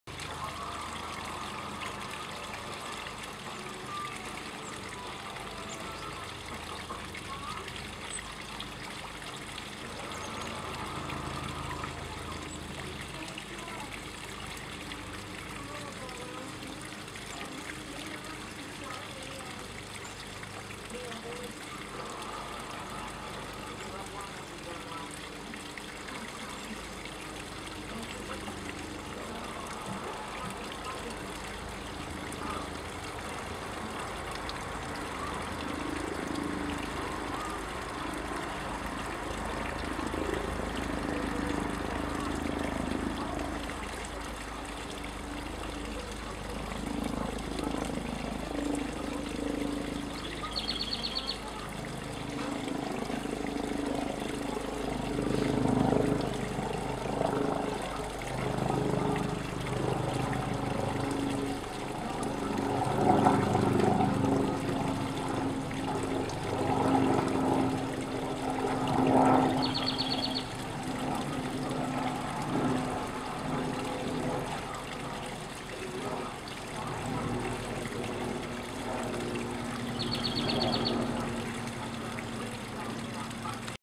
Hummingbird visiting our bird feeder
This hummingbird comes to our backyard feeder almost every 15-20 minutes in day time during the summer.  The feeder is right outside the window.  We have best view of seeing him dancing around the feeder.  Beautiful bird indeed.